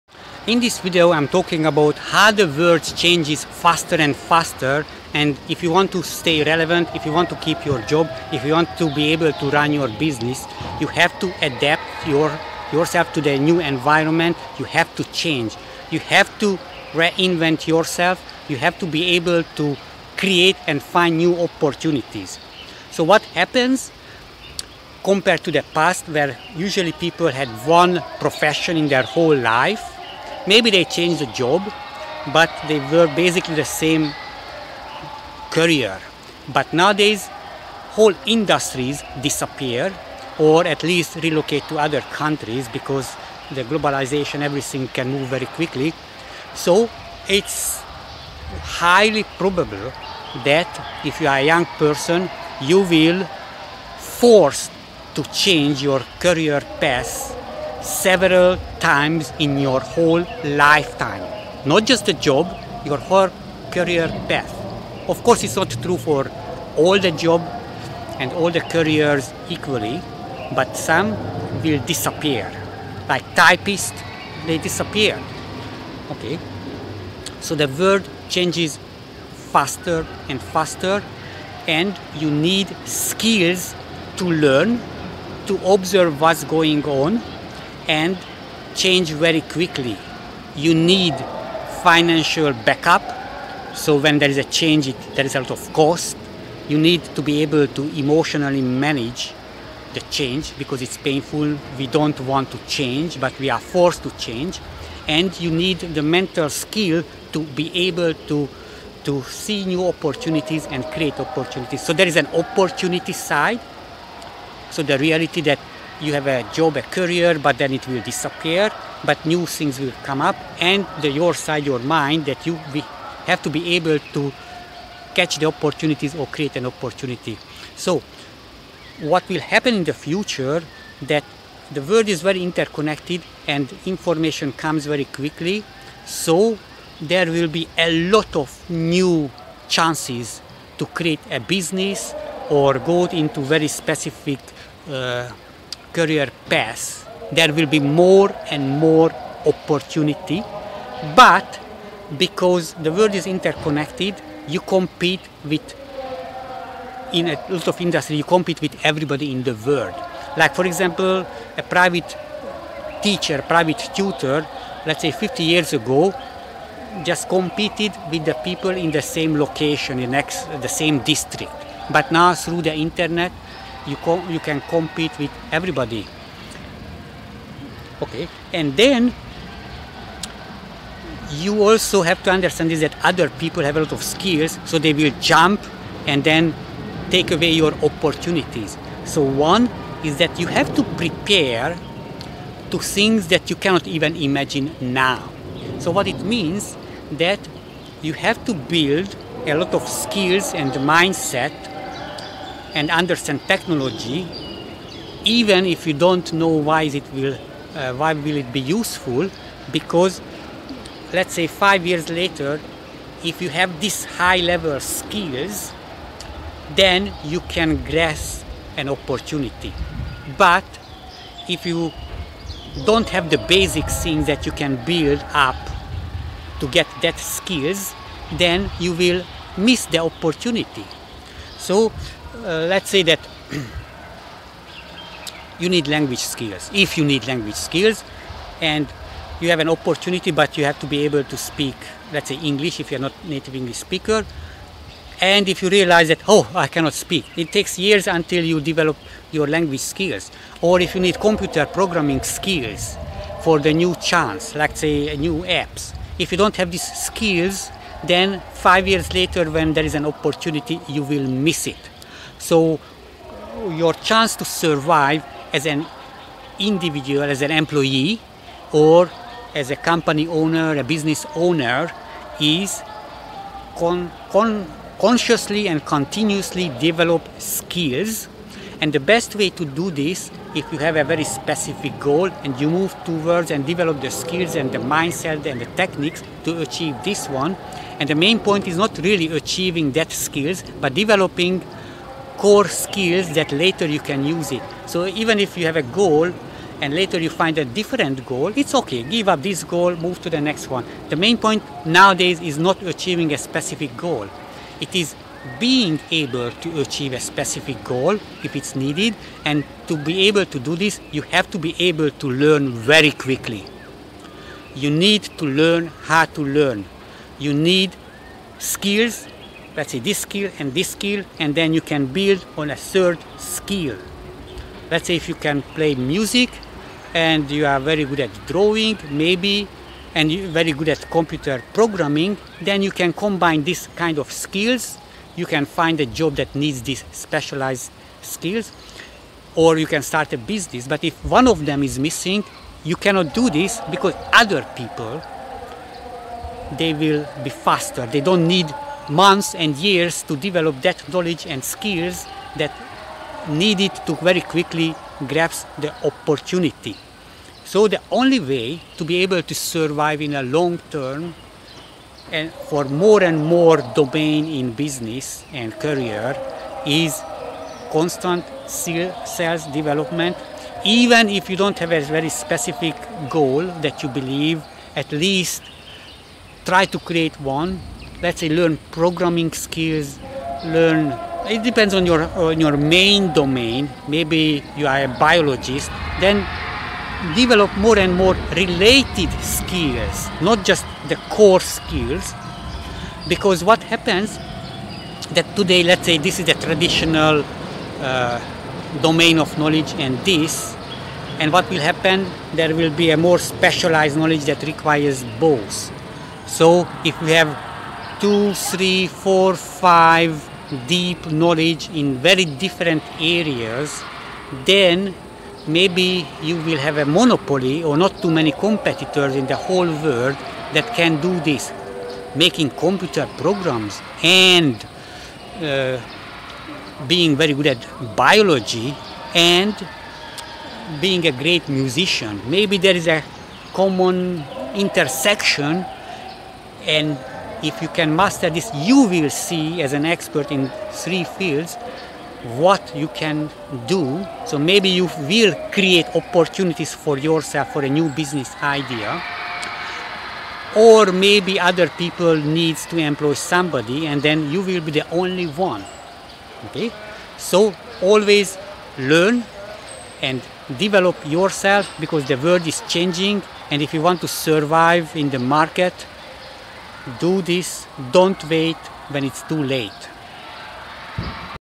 Develop skills to reinvent yourself, find & create new career opportunities in this changing world
As there are more people on Earth and we are more interconnected at very cheap price, innovative technology pop up every moment. Our world is changing in an accelerated speed. In our lifetime we will forced to change careers several times, not just jobs, because industries will disappear and you will be replaced by robots and computers or cheaper labor in other countries. Change and technological innovation means there will be more and more opportunities to succeed because more new things will emerge but because of the worldwide competition, there will be a short time to grab the opportunity. If you are not ready someone will get it faster. So what you can do is prepare for the future, learn how to learn faster, get new mindsets and skills, set goals and work on your goals - not necessary to achieve those goals but to learn new skills on the way and learn how to work on goals - then when a window of opportunity open, you may be ready to jump first before others.